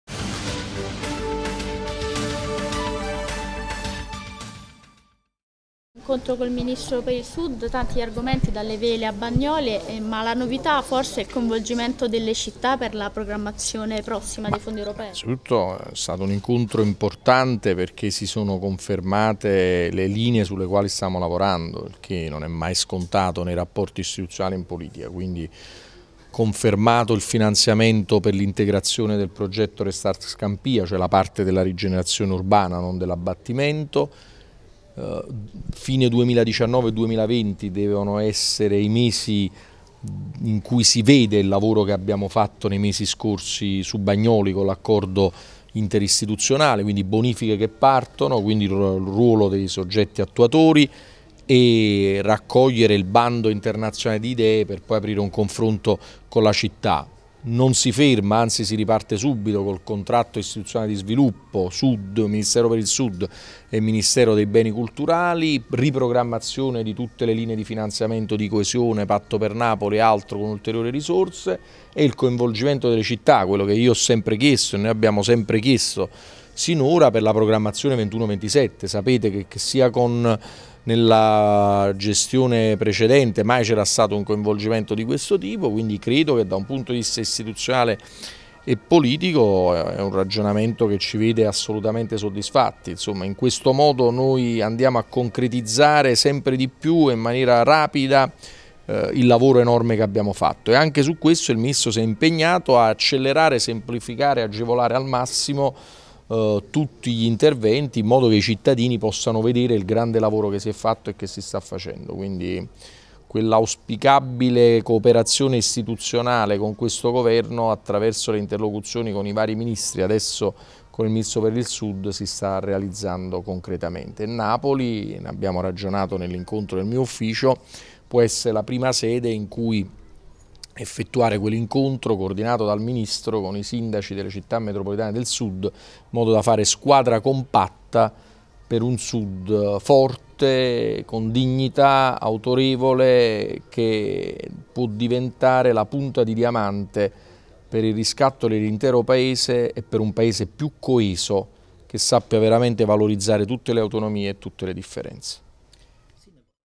De Magistris: Scampia, bonifica Bagnoli, patto per Napoli. Siamo soddisfatti
Una dichiarazione del sindaco di Napoli Luigi de Magistris al termine dell'incontro dell'8 ottobre 2019 col ministro per il sud e la coesione sociale Giuseppe Provenzano.